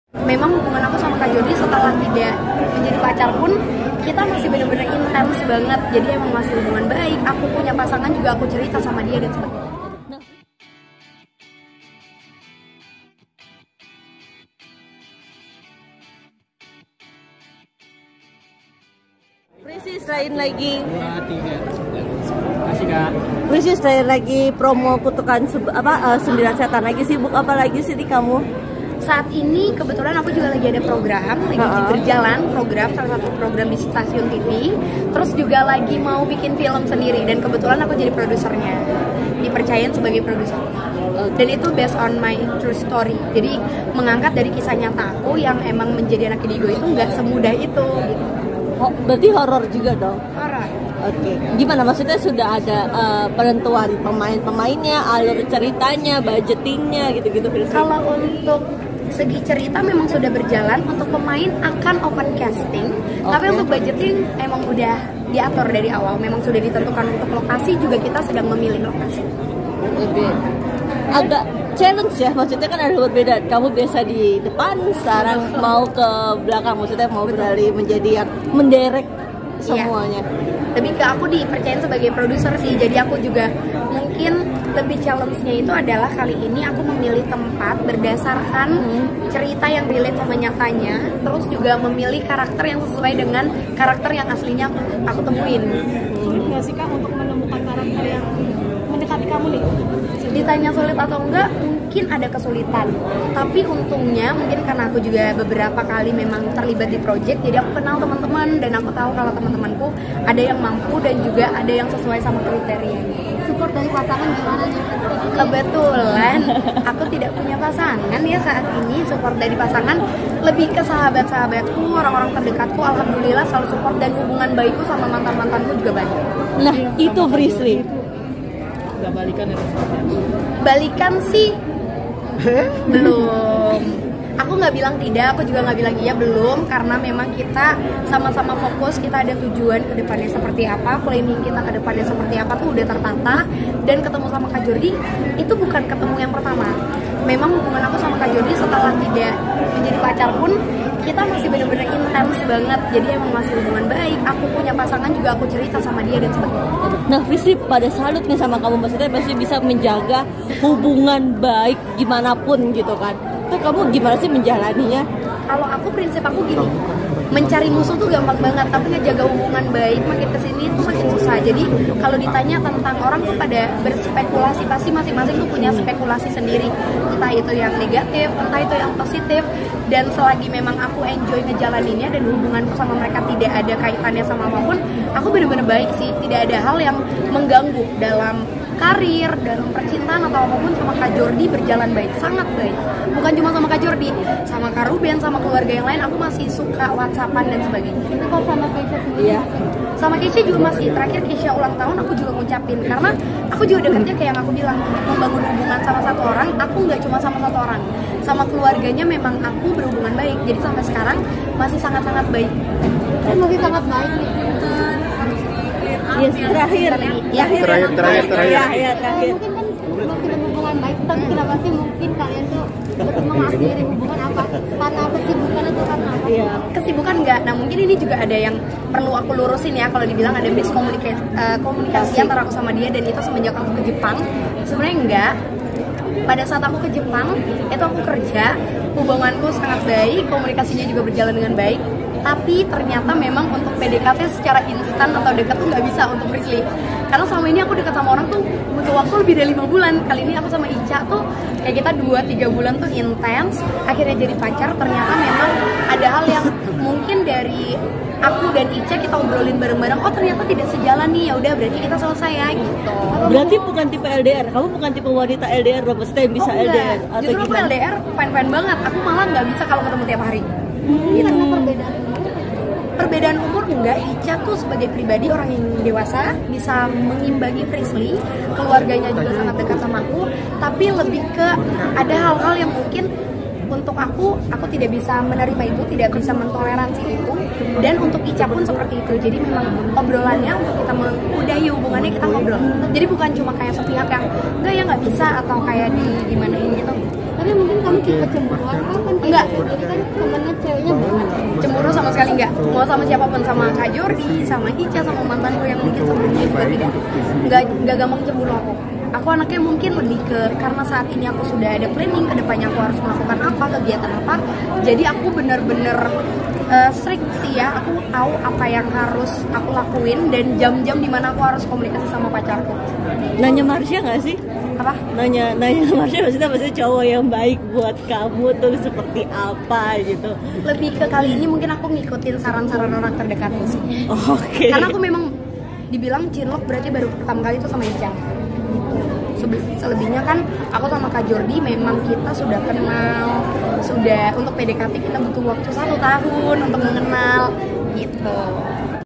FRISLLY HERLIND JELASKAN STATUSNYA DENGAN JORDI ONSU
Frislly Herlind tetap berhubungan baik dengan Jordi Onsu.